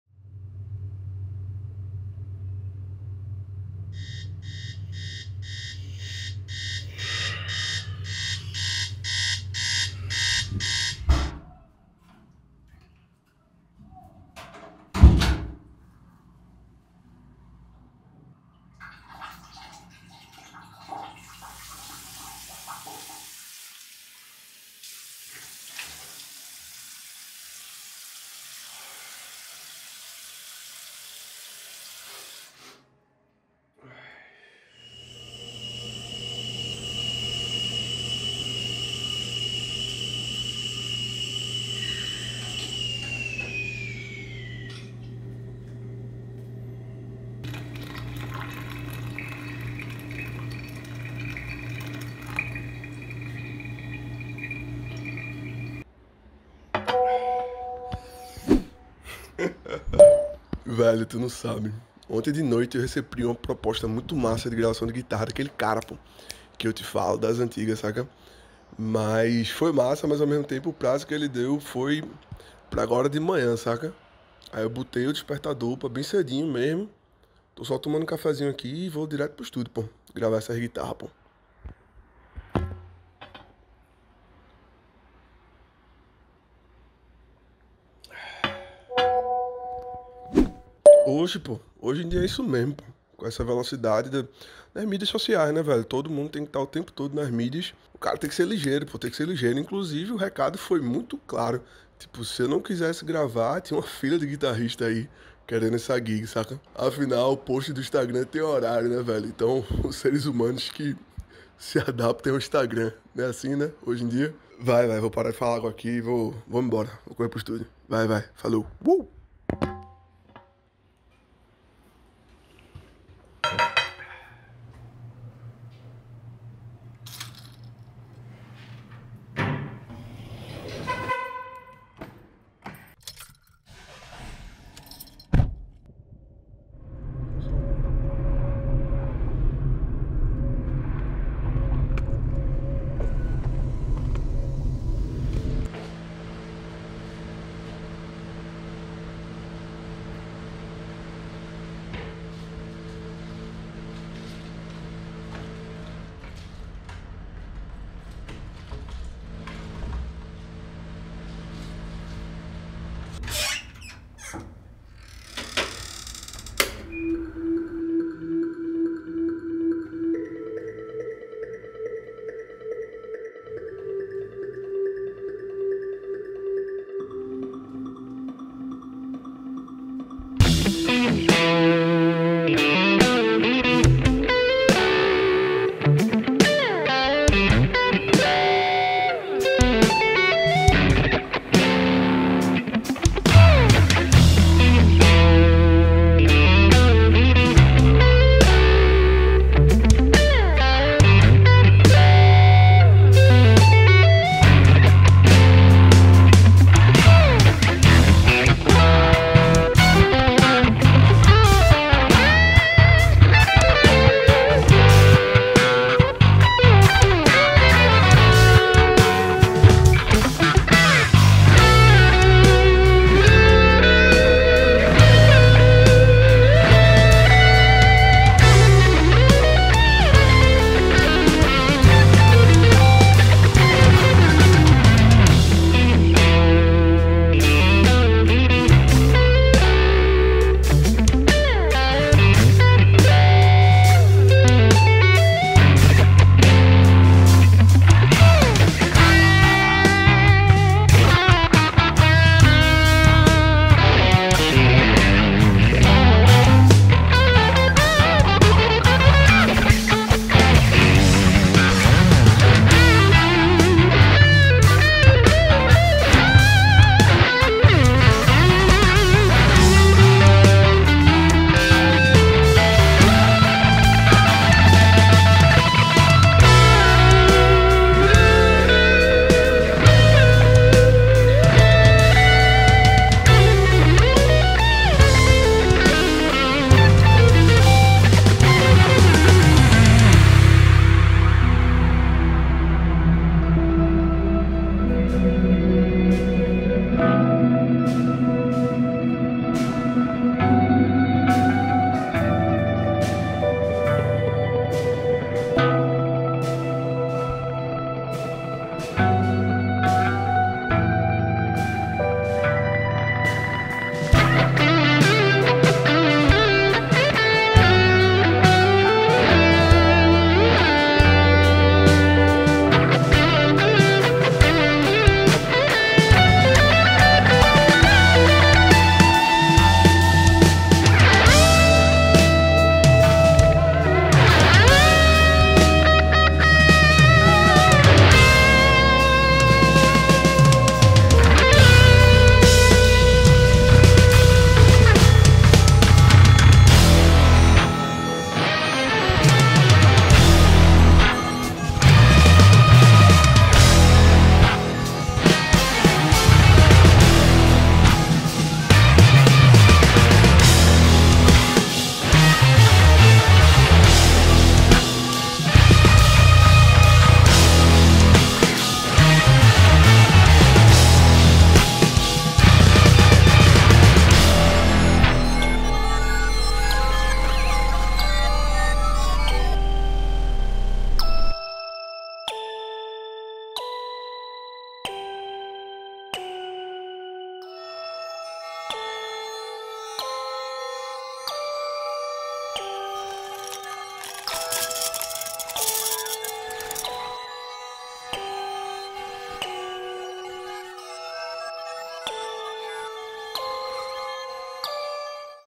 Analógico X Digital
"Analógico X Digital, Mundo Real ou Matrix, Vermelho X Azul, Pedal ou Pedaleira, QUAL TUA ESCOLHA?" é uma reflexão sobre o mundo extremamente VELOZ e imediatista em que vivemos.